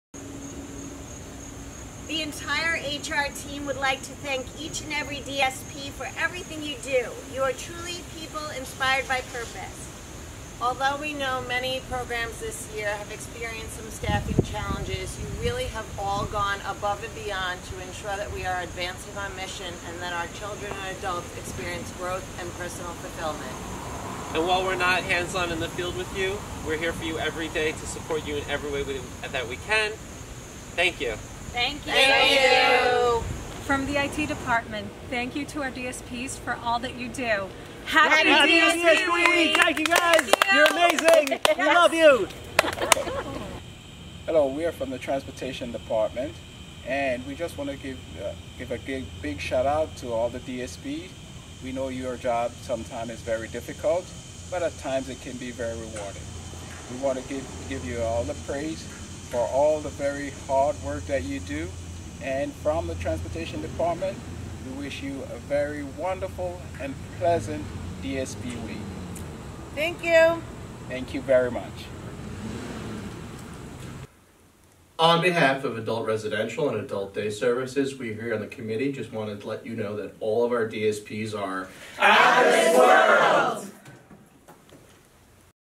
DSP Week Support Department Messages and DSP Week Committee
Watch special messages of thanks to our incredible DSPs from our Support Departments and the DSP Week Committee.